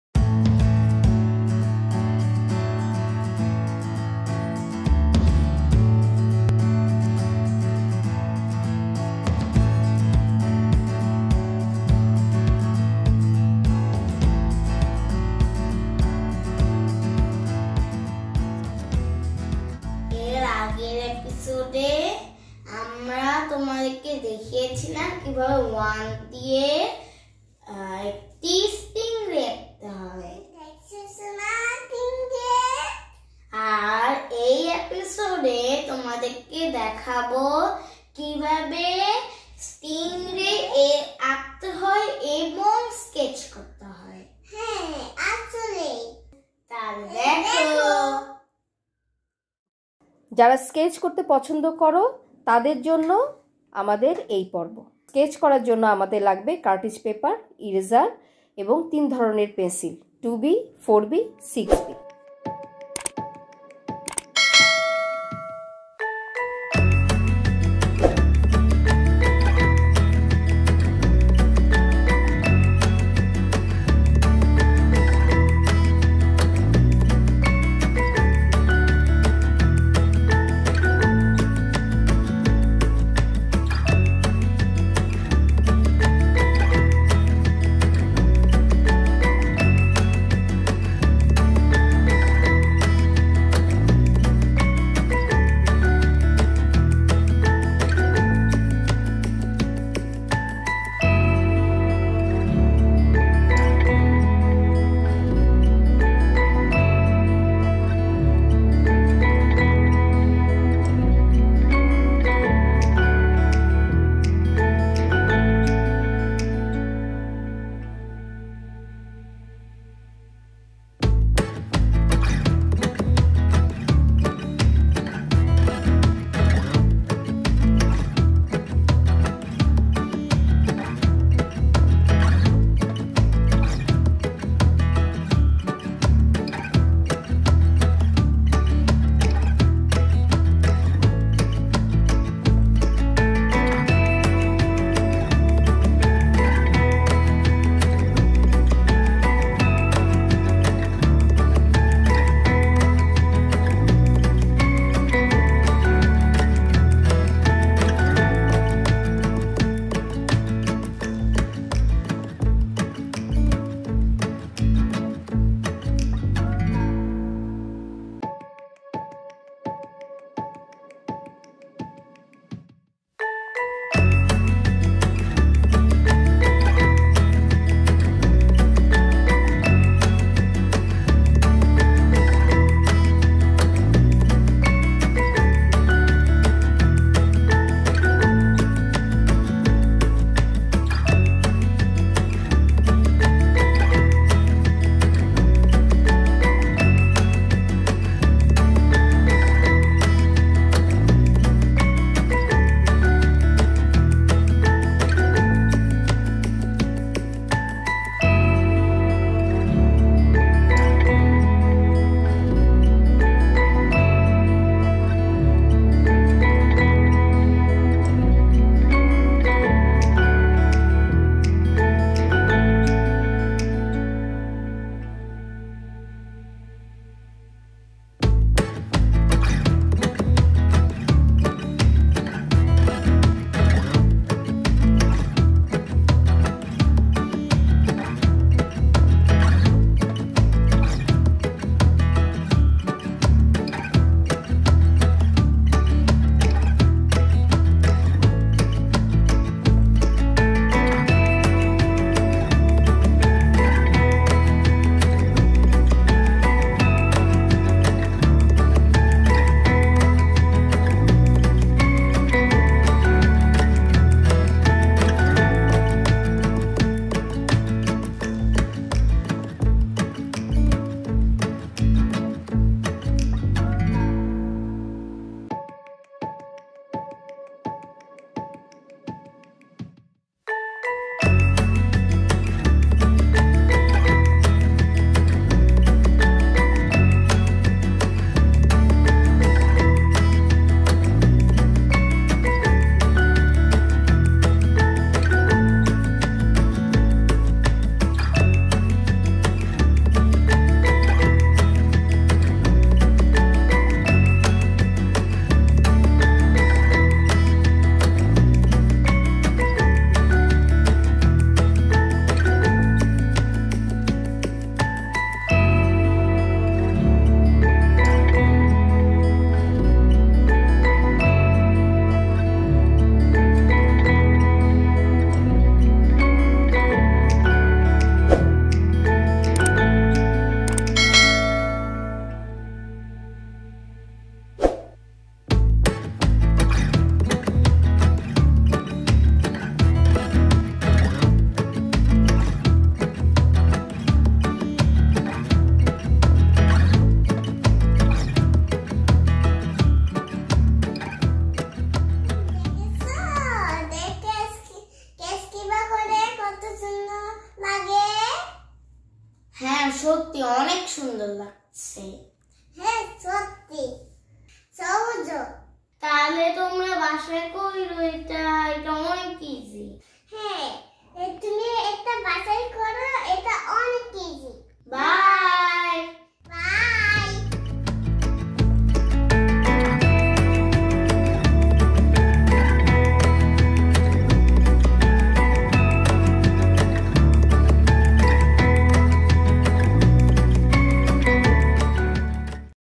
how to draw and sketch stingray( 2nd part)with 1কিভাবে খুব সহজে 1দিয়ে স্টিংরে আঁকা ও স্কেচ করা যায়
Out of The White Cube একটি উন্মুক্ত প্ল্যাটফর্ম ।আমরা সাধারণত সৃজনশীল বিষয় , পারিবারিক শিক্ষা, ভ্রমন, ইত্যাদি বিষয়কে প্রাধান্য দিয়ে কাজ করে থাকি।আমাদের এবারের আয়োজন, 1 দিয়ে কিভাবে খুব সহজে স্টিংরে আঁকা, রঙ করা এবং স্কেচ করা যায়  ।